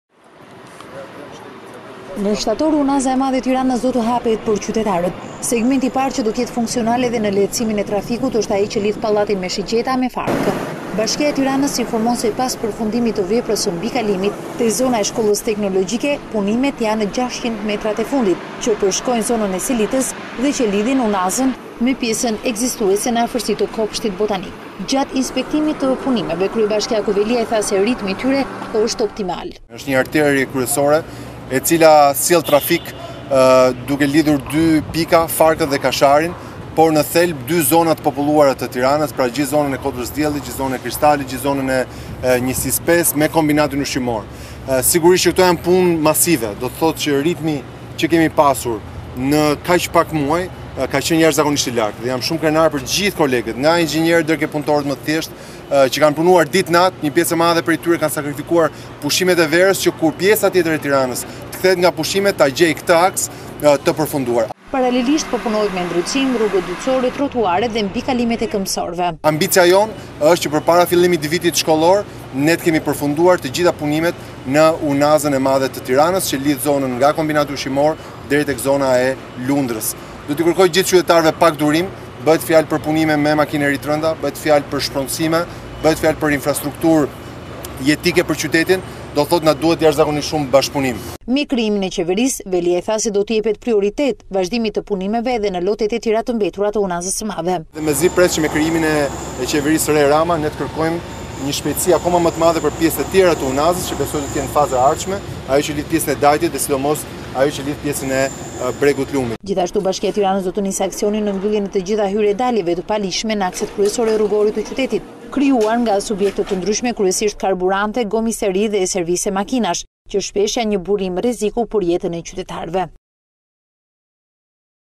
Ora News - Hapet në shtator Unaza e Madhe. Ja si duket sot
Në shtator Unaza e Madhe e Tiranës do të hapet për qytetarët. Segmenti i parë që do të jetë funksional edhe në lehtësimin e trafikut është ai që lidhen Pallatin me Shigjeta me Farkën.
Bashkia e Tiranës informon se pas përfundimit të veprës së mbikalimit te zona e Shkollës Teknologjike, punimet janë në 600 metrat e fundit që përshkojnë zonën e Selitës dhe që e lidhin Unazën me pjesën ekzistuese në afërsi të Kopshtit Botanik.  Gjatë inspektimit të punimeve, kryebashkiaku Veliaj tha se ritmi i punimeve është optimal.
Veliaj: “Ky është një projekt jetik për qytetin, një arterie kryesore e cila sjell trafik, duke lidhur dy pika, Farkën dhe Kasharin, dy zona të populluara të Tiranës, pra gjithë zonën e Kodrës së Diellit, Kristalit, Njësinë 5 me Kombinatin Ushqimor. Sigurisht, ky është një projekt i madh, që do të thotë se edhe ritmi që kemi pasur në kaq pak muaj ka qenë jashtëzakonisht i lartë, ndaj jam shumë krenar për të gjithë kolegët, nga inxhinierët deri tek punëtorët më të thjeshtë, që kanë punuar ditë e natë, një pjesë e madhe prej tyre kanë sakrifikuar pushimet e verës, që kur pjesa tjetër e Tiranës të kthehet nga pushimet, ta gjejë këtë aks të përfunduar”
Për më shumë lexoni në: